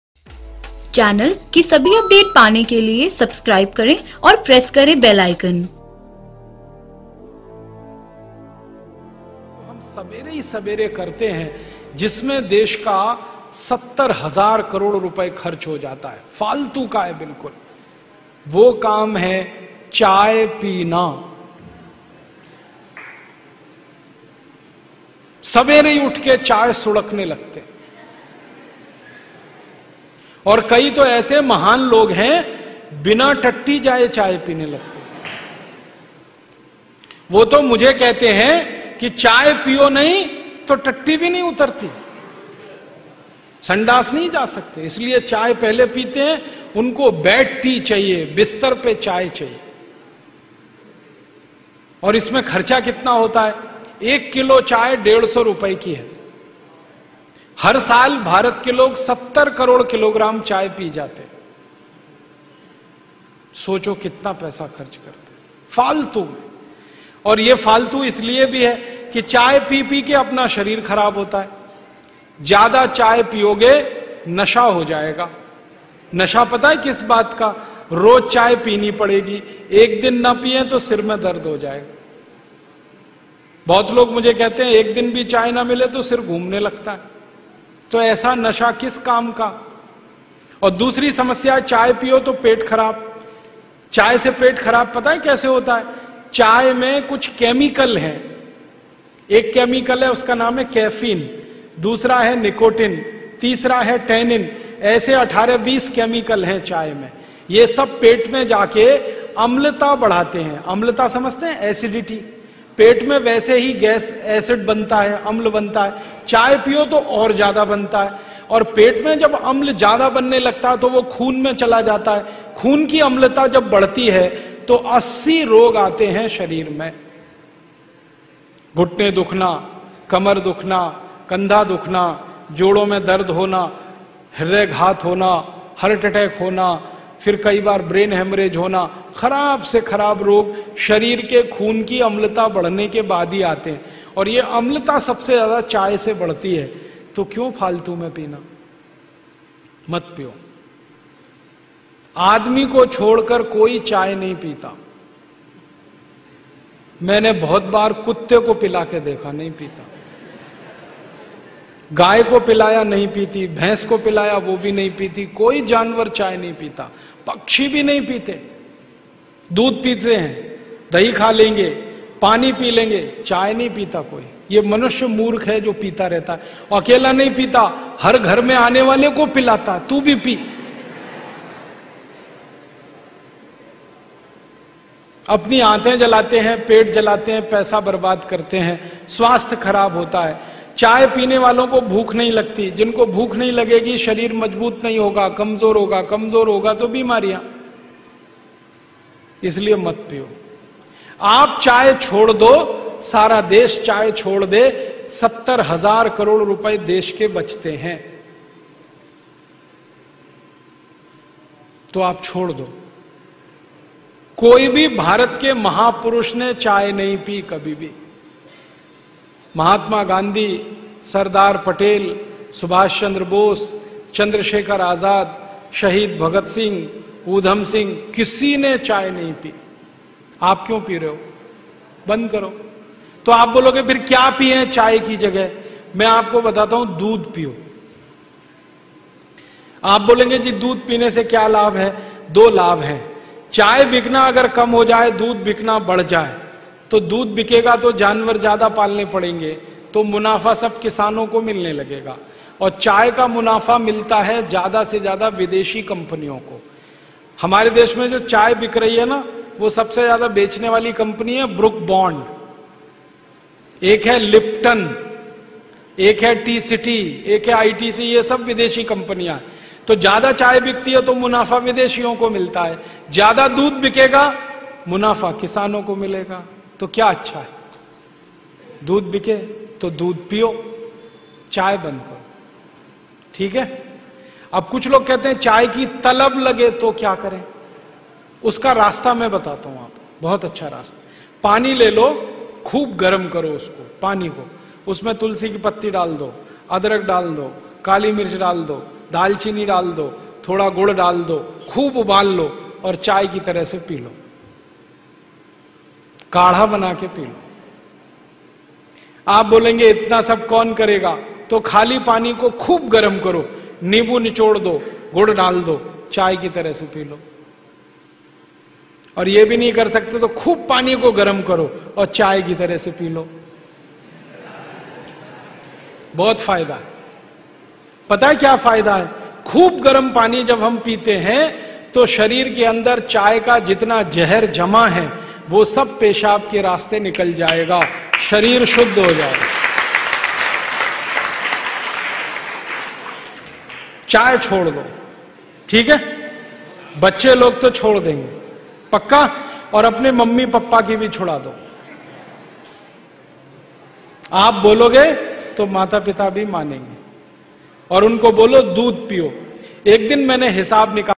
चाय पीने के 80 नुकसान | Chaay peene se nuksaan | side effect of tea in hindi by Rajiv Dixit ji
इस वीडियो में राजीव भाई  ने चाय पीने से होने वाले 80 नुकसानों के बारे में बताया है, सभी चाय पीनी वाले भाई और बहने इसे जरूर सुने